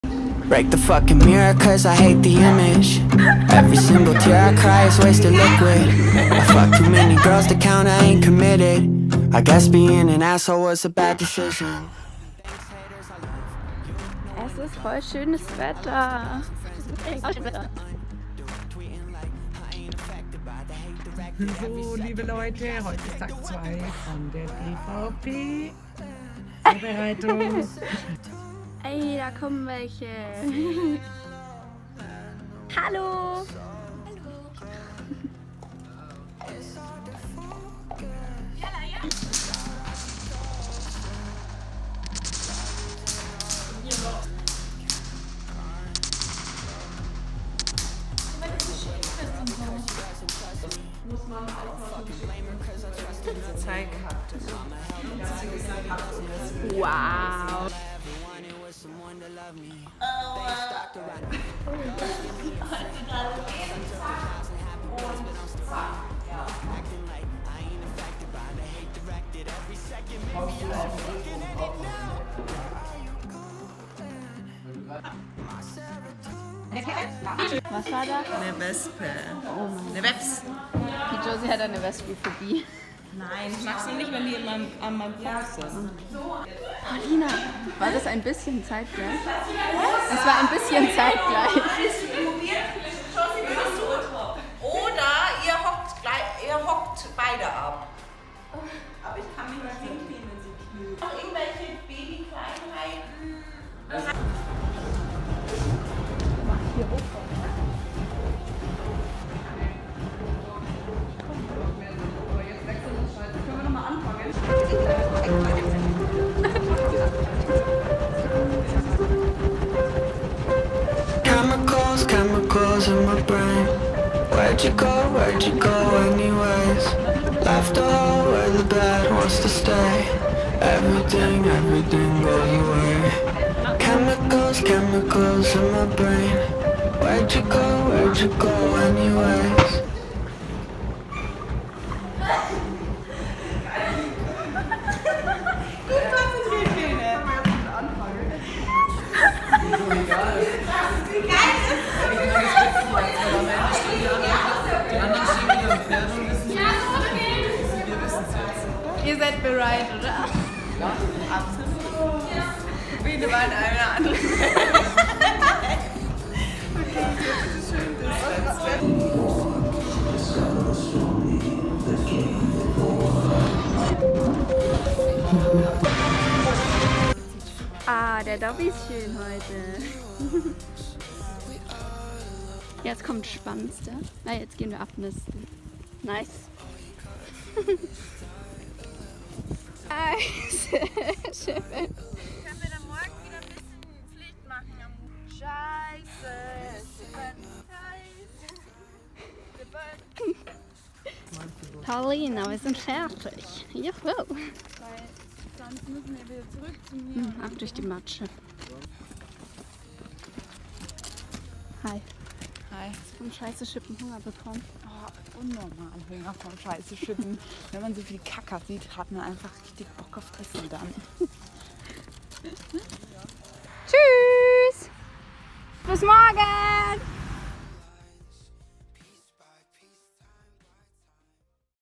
Oberland 1 | Vorbereitung Deutscher Voltigierpokal 2019 (Teil 2)
RVF Oberland 1
LK M**
Vorbereitungs-Tagebuch
Deutscher Voltigierpokal Zweibrücken 2019
Teil 2
Video-Blog
Training
Movietraining
Samstag, 12. Oktober 2019